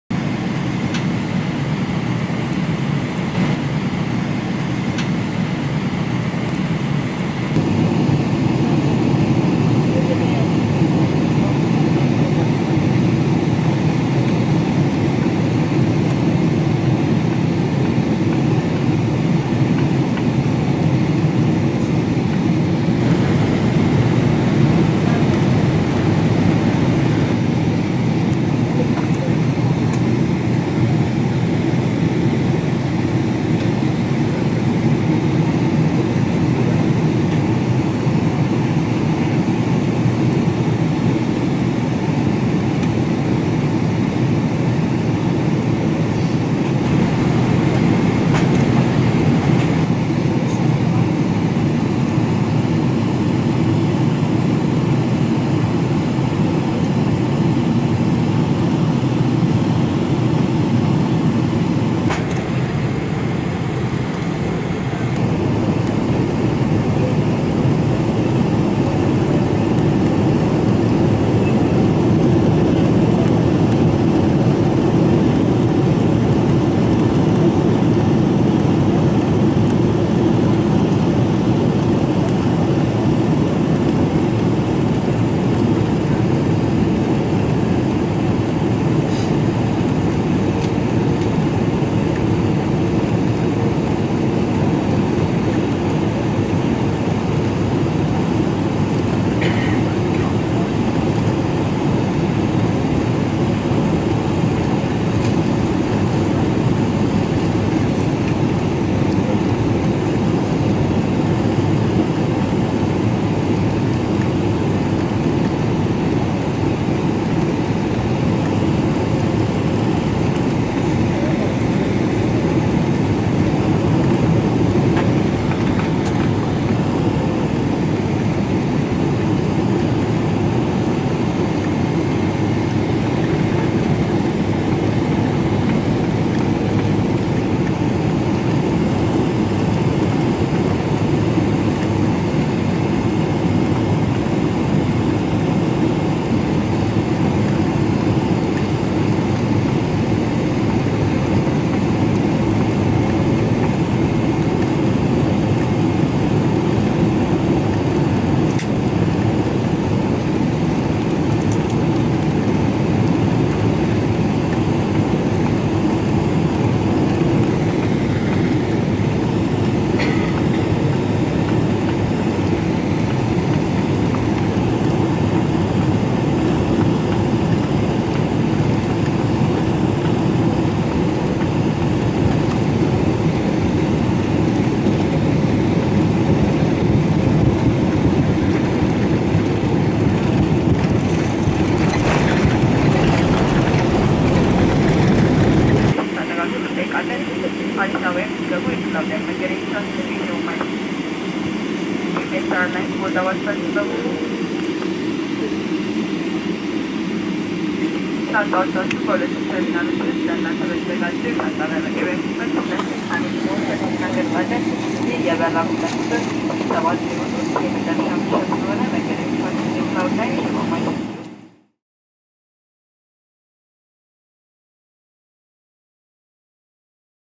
Landing/atterraggio in Addis Ababa, Flight Ethiopian ET 703 Fco-Add
At window/dal finestrino
Ethiopian Airlines, ET 703 Boeing 777-200 LR
Roma FCO (ITALIA) - Addis Ababa ADD (Ethiopia)
Landing/atterraggio/landen in Addis Ababa